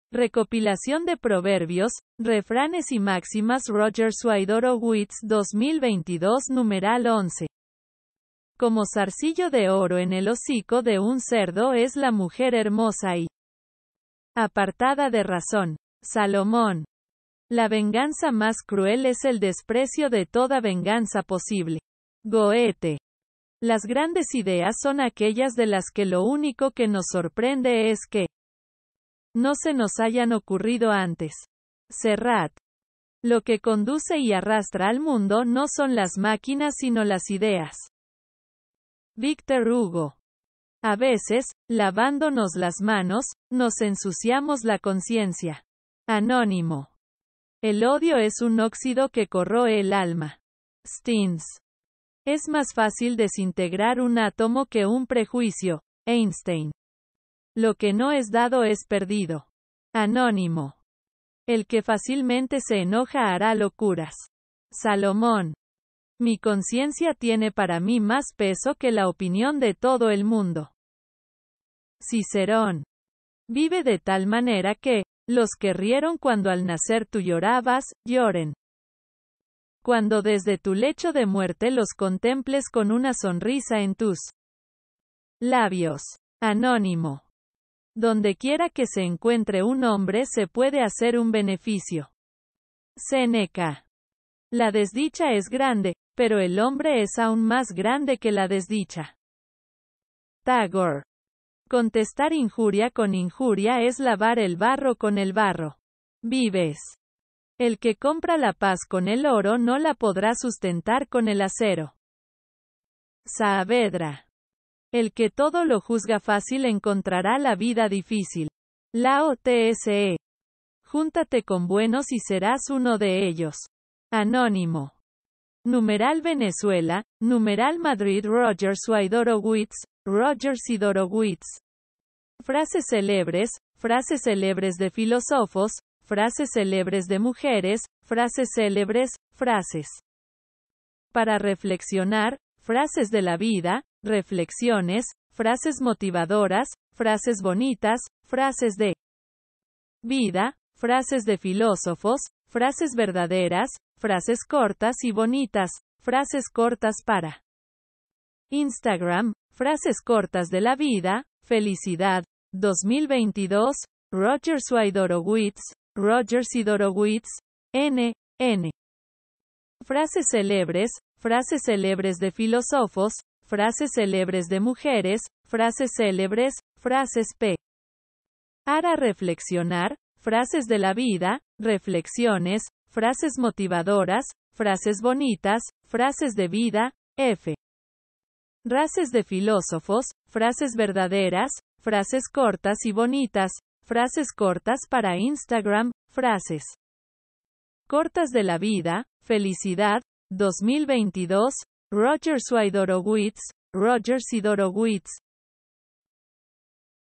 Recopilación de proverbios, refranes y máximas Roger Swidorowicz 2022 #11
-Como zarcillo de oro en el hocico de un cerdo es la mujer hermosa y
apartada de razón. (Salomón)
-La venganza más cruel es el desprecio de toda venganza posible. (Goethe)
-Las grandes ideas son aquellas de las que lo único que nos sorprende es que
no se nos hayan ocurrido antes. (Serrat)
-Lo que conduce y arrastra al mundo no son las máquinas sino las ideas.
(Victor Hugo)
-A veces, lavándonos las manos, nos ensuciamos la conciencia. (Anónimo)
-El odio es un óxido que corroe el alma. (Steens)
-Es más fácil desintegrar un átomo que un prejuicio.(Einstein)
-Lo que no es dado es perdido. (Anónimo)
-El que fácilmente se enoja hará locuras. (Salomón)
-Mi conciencia tiene para mí más peso que la opinión de todo el mundo.
(Cicerón)
-Vive de tal manera que , los que rieron cuando al nacer tu llorabas, lloren,
cuando desde tu lecho de muerte los contemples con una sonrisa en tus
labios. (Anónimo)
-Dondequiera que se encuentre un hombre se puede hacer un beneficio.
(Séneca)
-La desdicha es grande, pero el hombre es aun más grande que la desdicha.
(Tagore)
-Contestar injuria con injuria es lavar el barro con el barro. (Vives)
-El que compra la paz con el oro no la podrá sustentar con el acero.
(Saavedra)
-El que todo lo juzga fácil encontrará la vida difícil. (Lao Tse)
-Júntate con buenos y serás uno de ellos. (Anónimo)